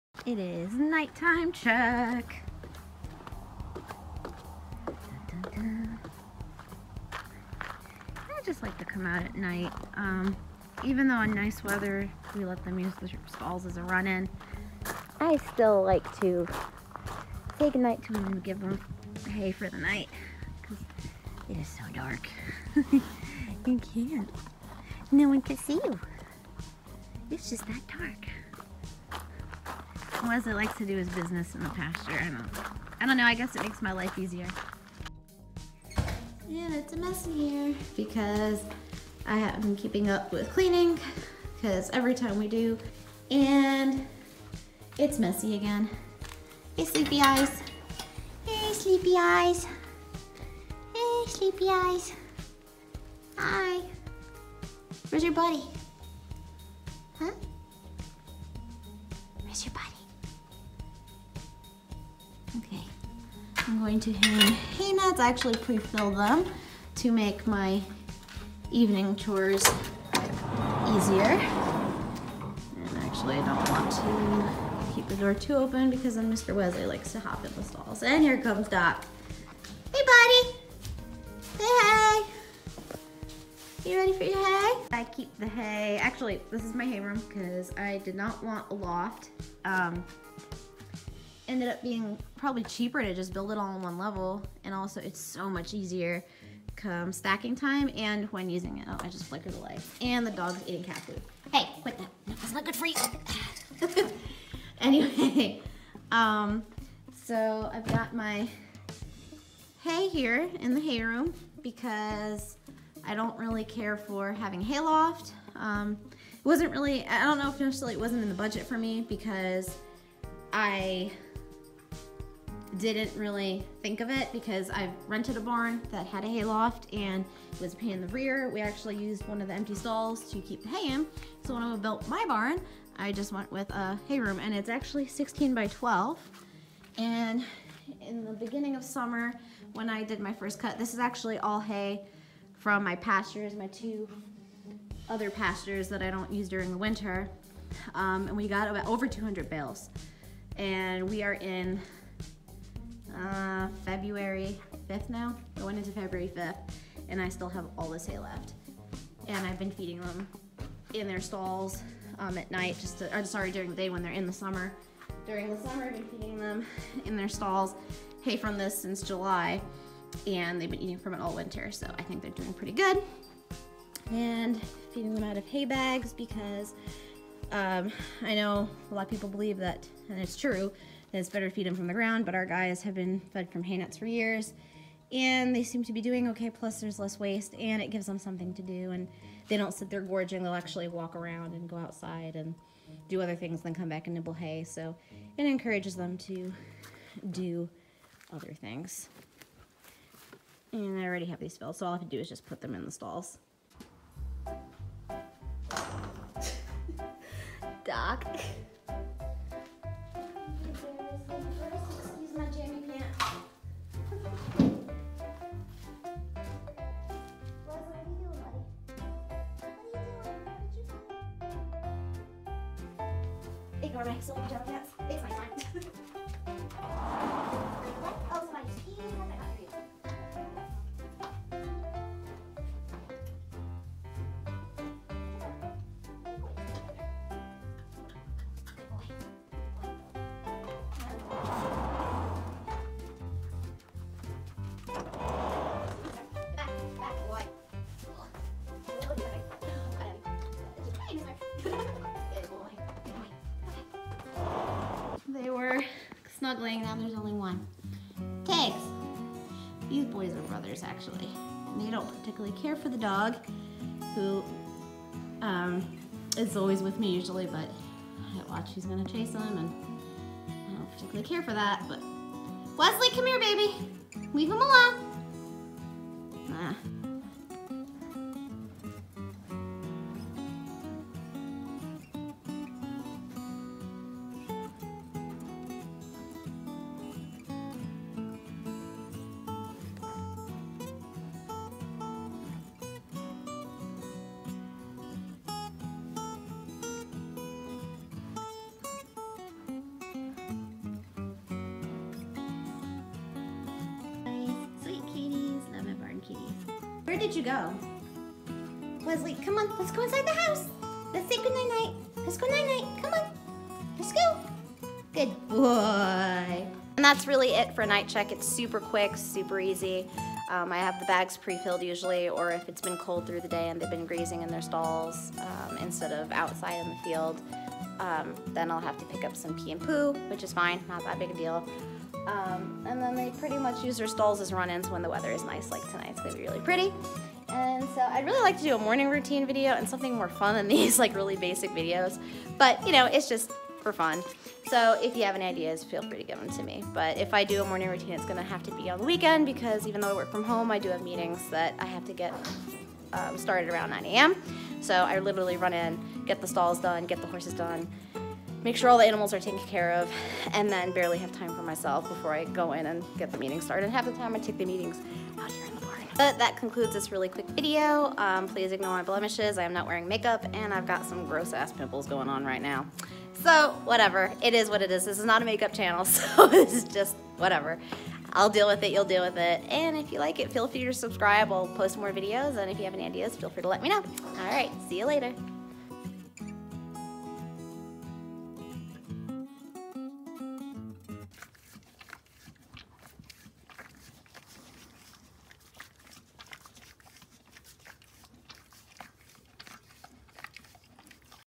BARN VLOG // Winter Night-Time Check
I definitely want to do more diverse videos, but felt like since I had a a winter evening feed routine, it was fitting to add my winter nighttime check routine.

Feel free to suggest any new types of videos!